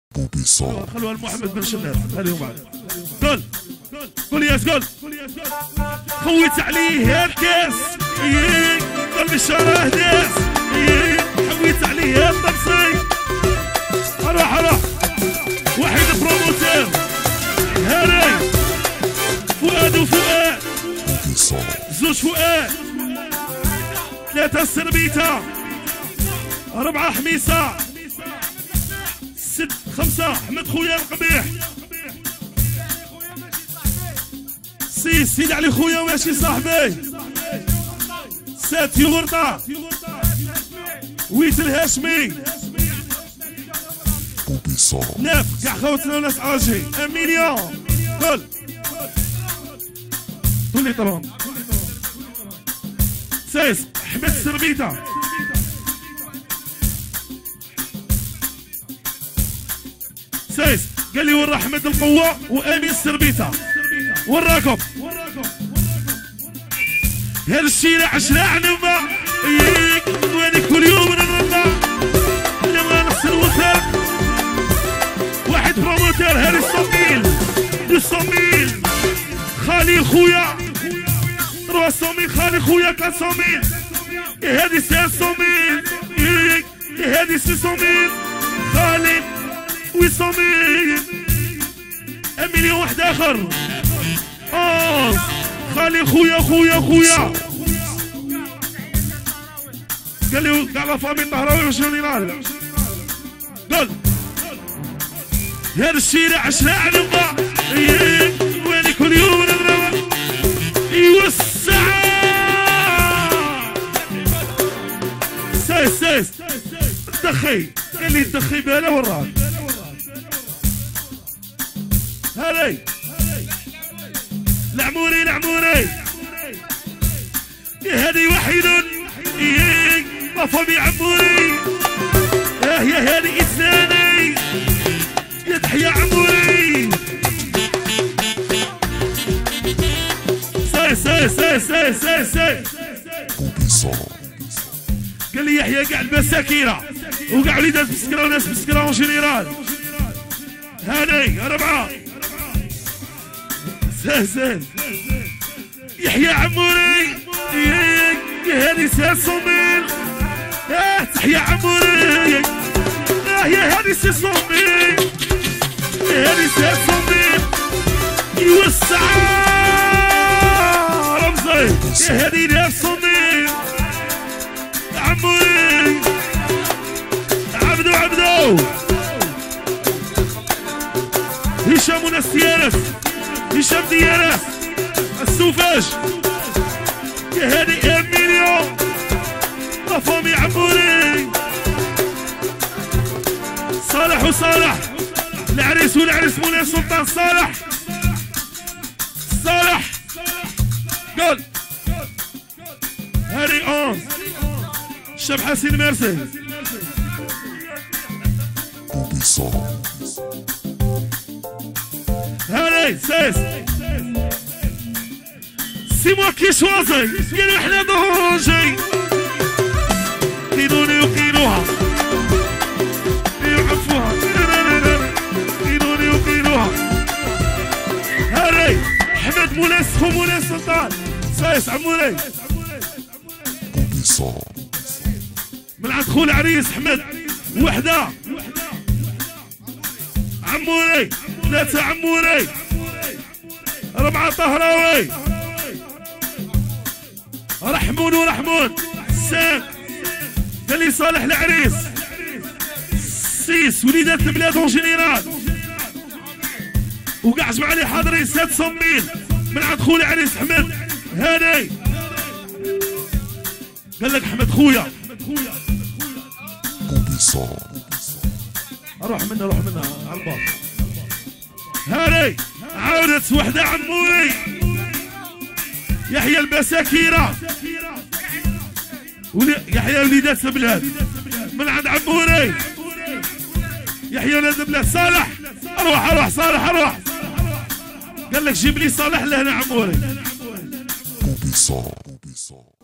Cheb Hassine 06 mariage SALAH RAHMON LIVE BISKRA BOUBI SON By •••►DJ HASNI
LIVE BISKRA BOUBI SON
      Mariage SALAH RAHMON
           Studio  Boubi Son
▶️ Cheb Hassine
▶️ Pianiste : LIASE LFAR
▶️ Perc : RAFIK LMAKNIN
▶️ Mix : Dj Hasni
▶️ Distributeur Digital : Sido SL